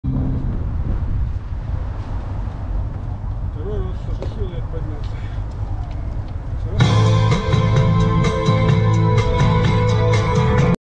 2022-10-07 16-36 (08) VicIVA + ValeSTAR Dancing acro Exhibition Парк Горького хастл, МУМИЙ ТРОЛЛЬ.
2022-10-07, Пятница,  VicIVA + ValeSTAR Dancing acro Exhibition Парк Горького хастл, МУМИЙ ТРОЛЛЬ.